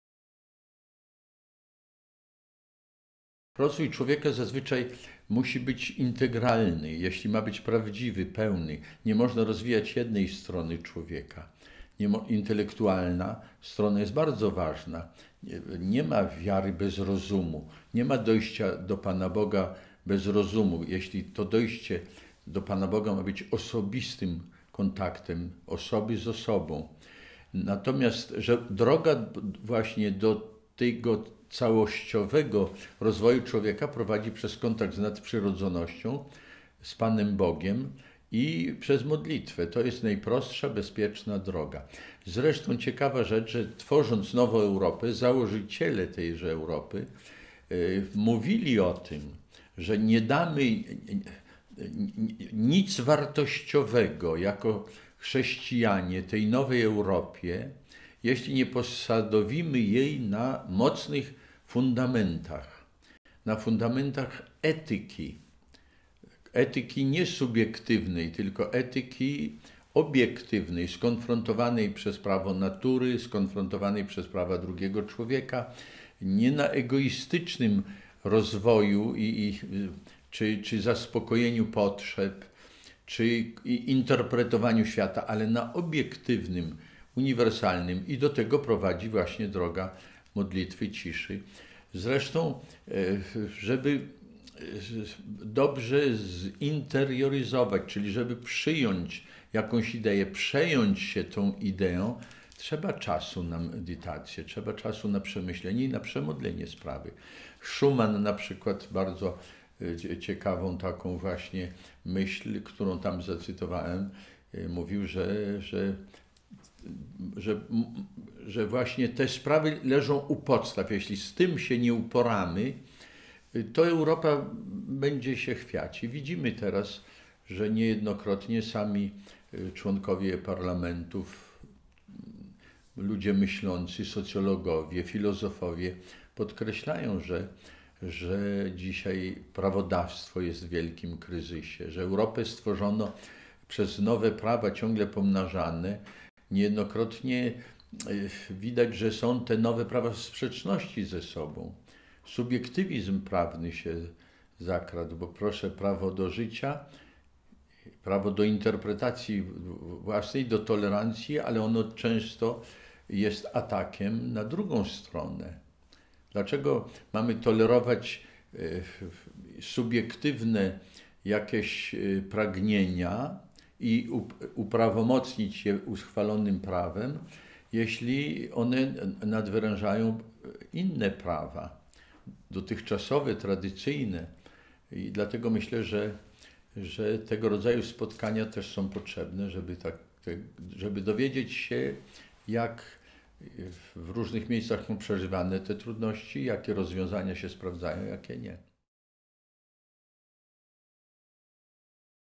Problemy i wyzwania wiary współczesnej Europy cz.2
Abp Józef Michalik po zgromadzeniu plenarnym Rady Konferencji Biskupich Europy (CCEE) opowiada o problemach i wyzwaniach współczesnej Europy dotyczących wiary. Obrady rozpoczęły się w czwartek 28 września w Sankt Gallen w Szwajcarii.